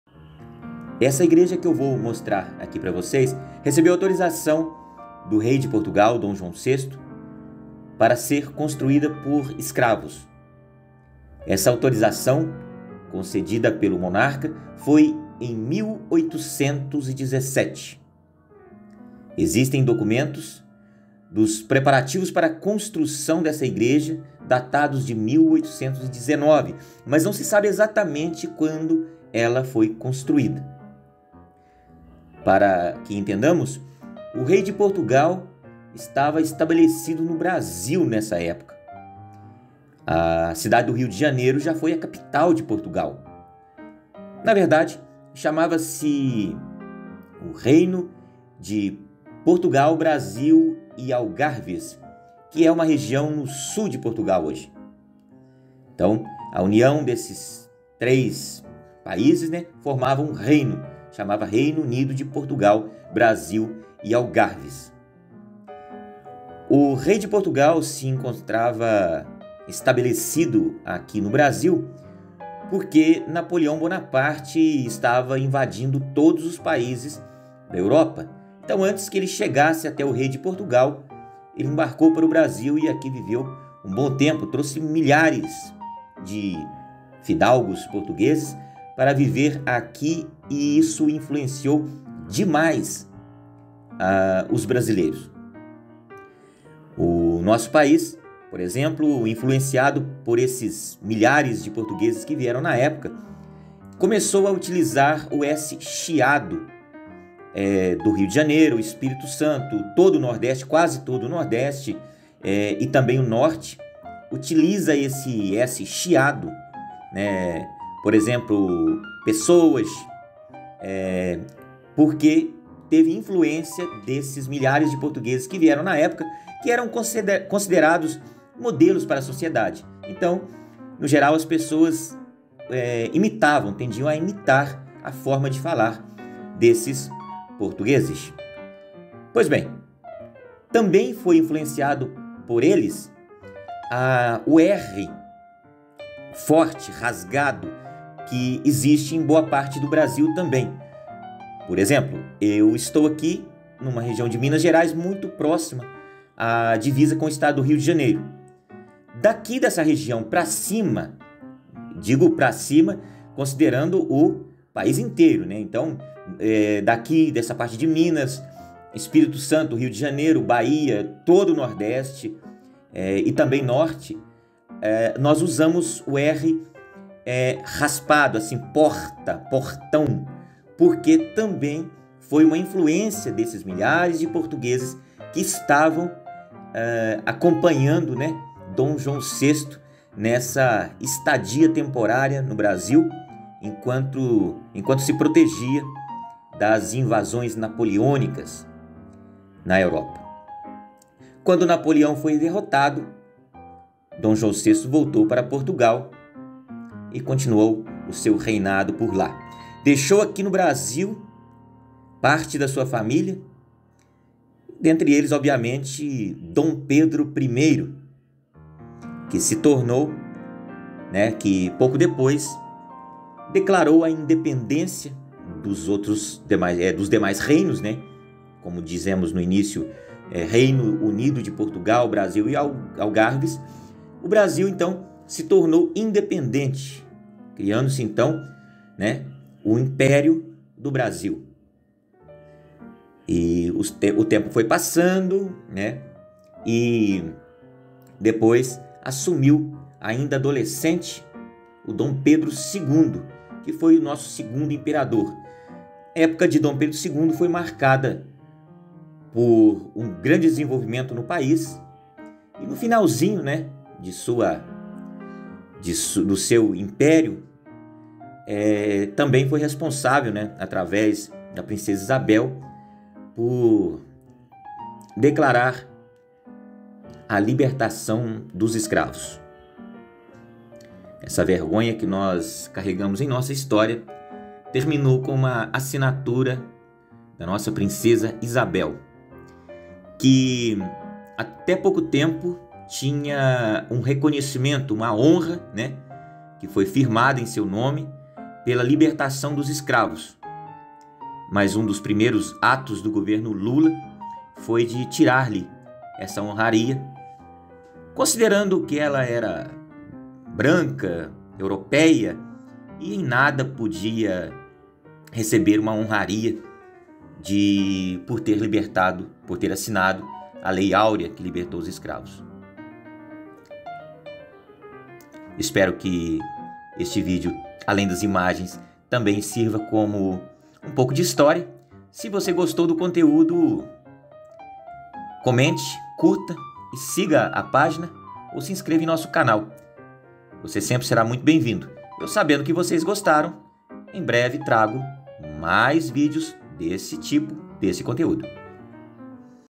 Você sabia que a capital de Portugal foi no Brasil?
Lula tira medalha de honraria à princesa Isabel por ter libertado os escravos e dá para Luiz Gama.